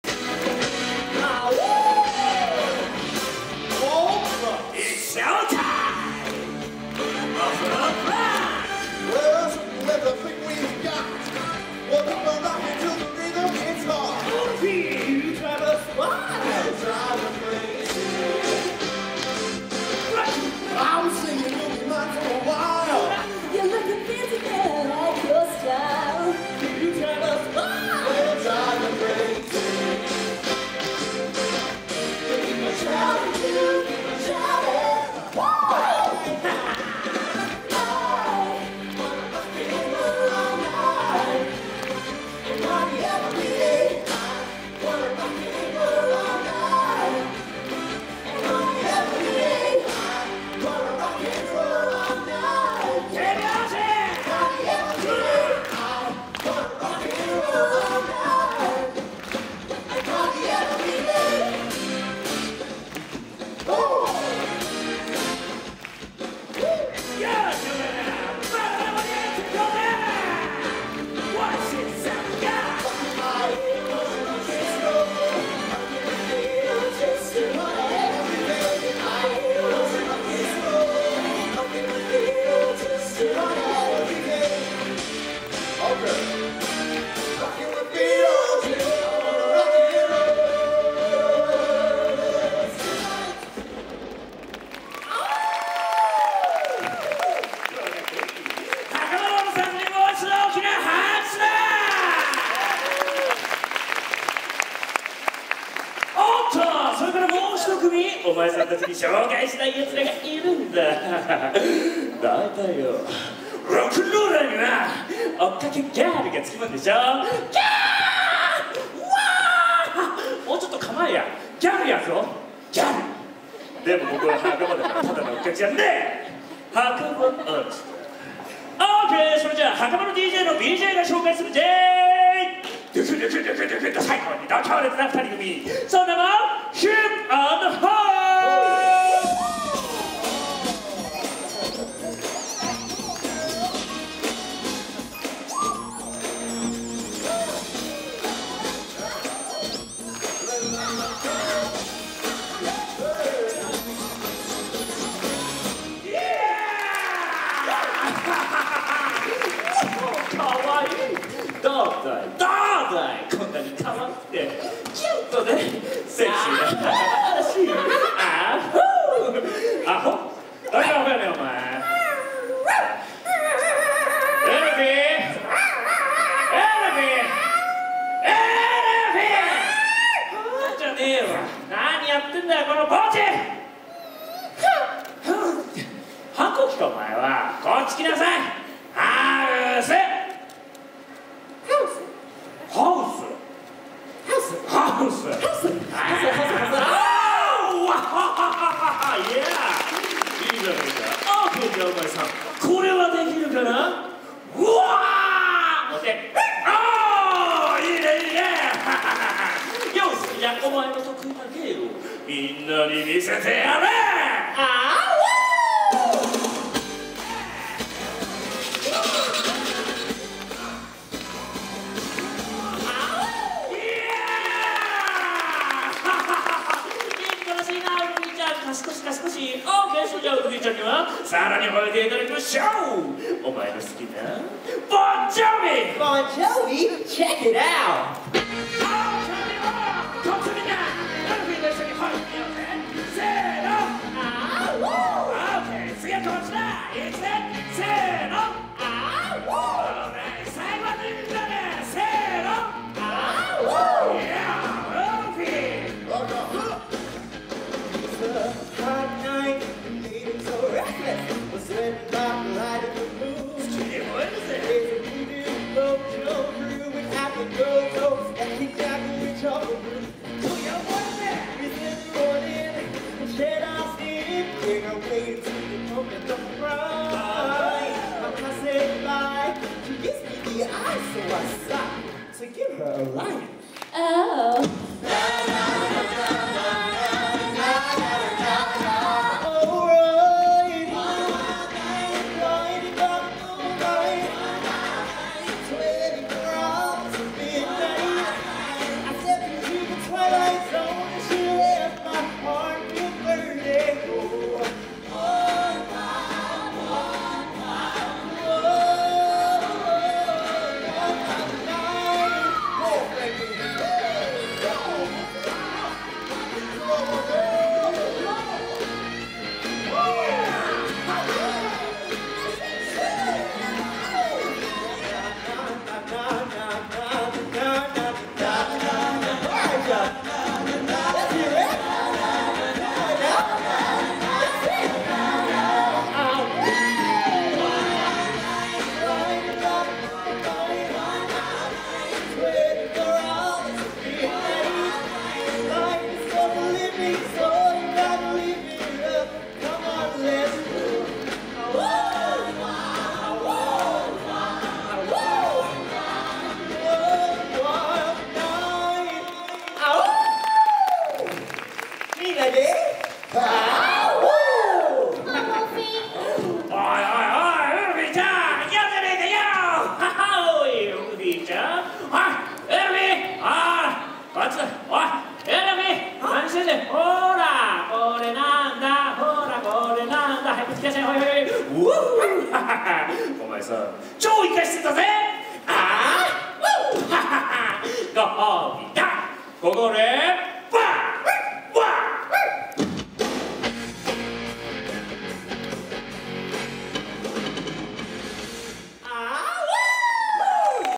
USJ 綺麗なダンスで魅了するブレットウルさん2017.2.3
ユニバーサル・モンスターズ・ライブ・ロックンロール・ショー
2/3三回目のたっくんBJさんとブレットウルさんの絡みも面白いぞ！